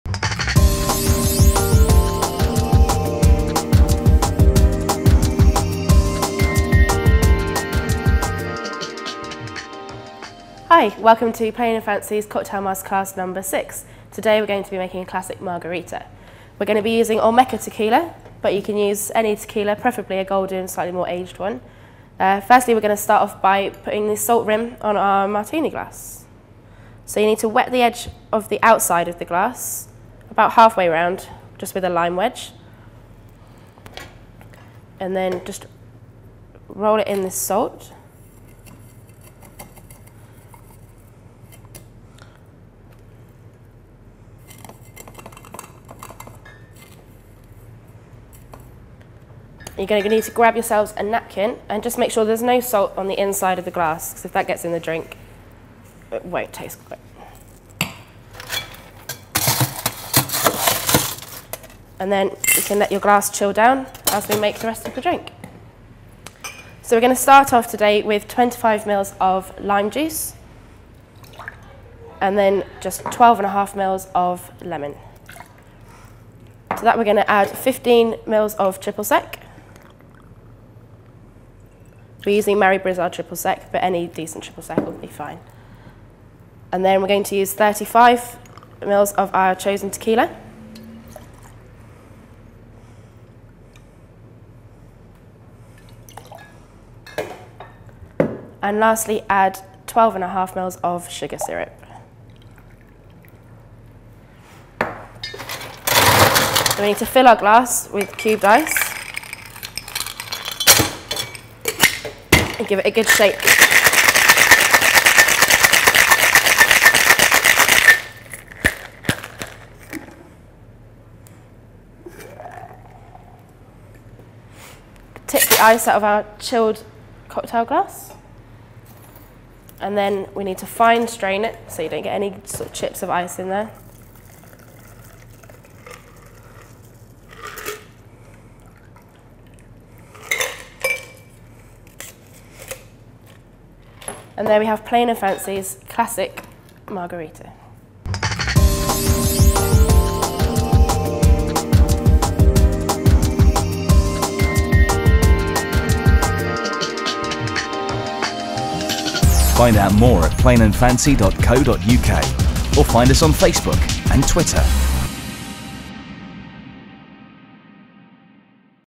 How to make a Classic Margarita
How to make a Classic Margarita courtesy of Winchester's No1 Cocktail Bar Plain and Fancy.